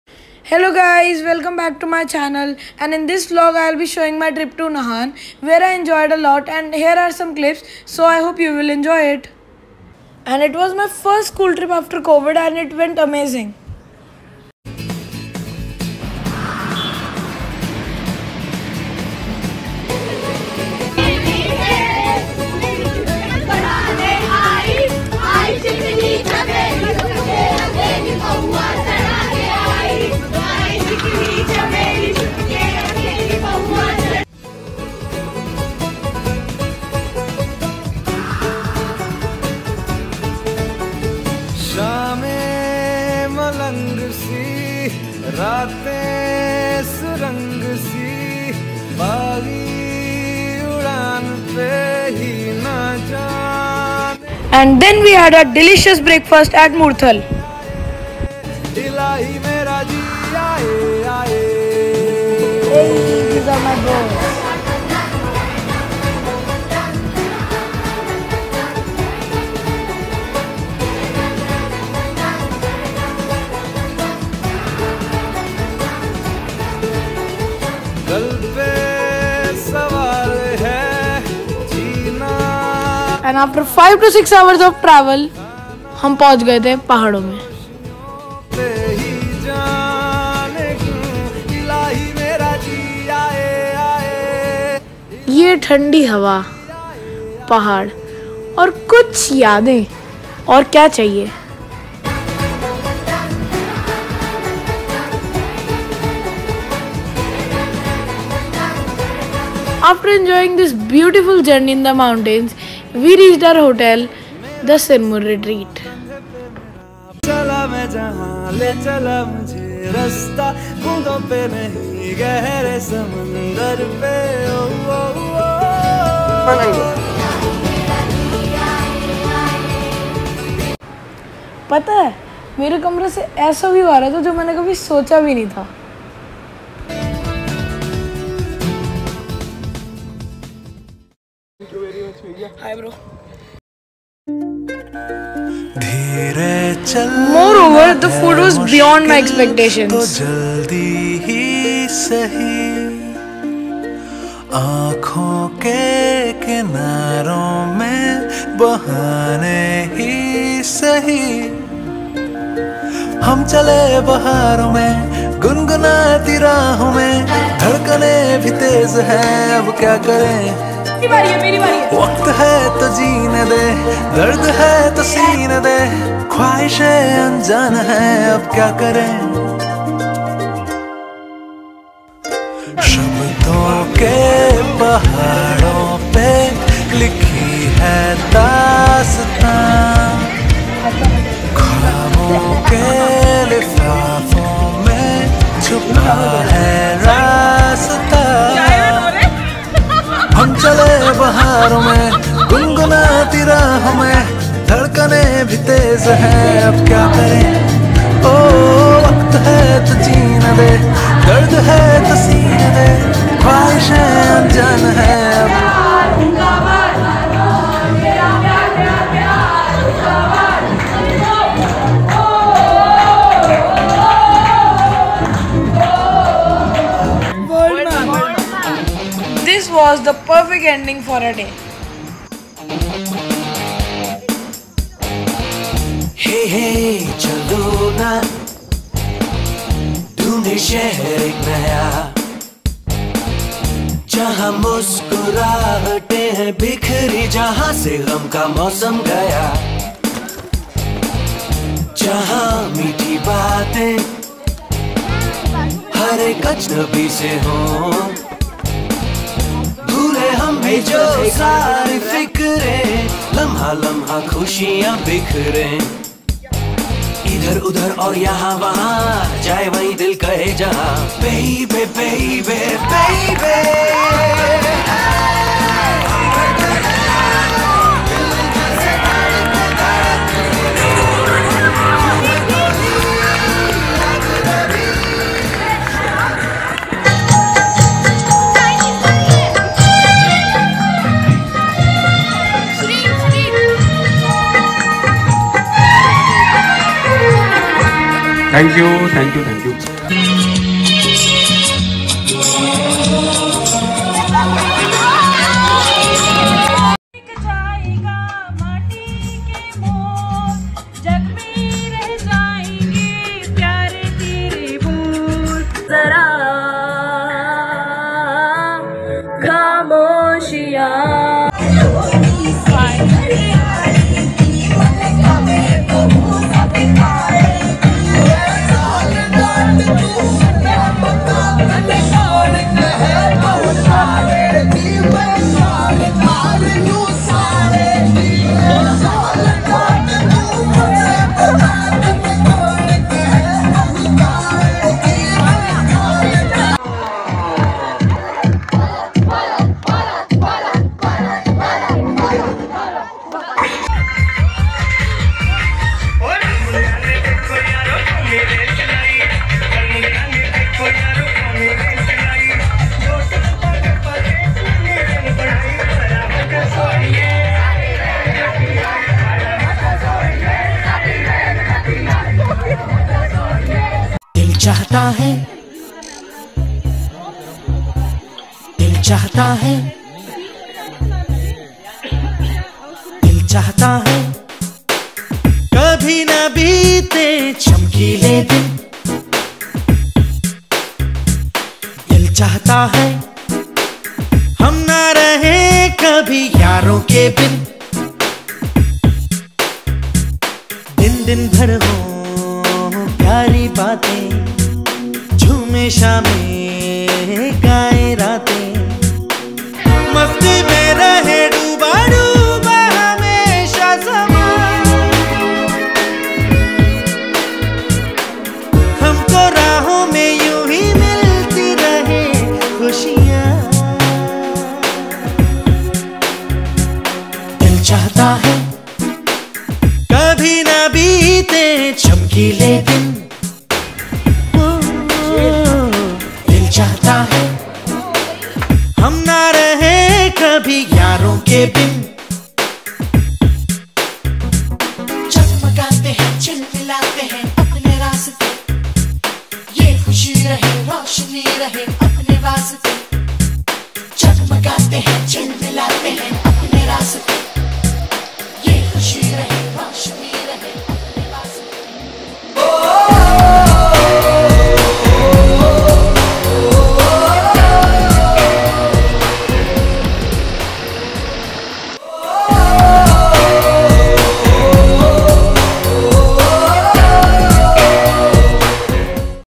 Trip to Nahan | Vlog | Apeejay School, Pitampura | Himachal Pradesh | Dishen Gupta
Hey guys!
I am Dishen Gupta and am passionate about dancing and acting 🎭 🕺🏻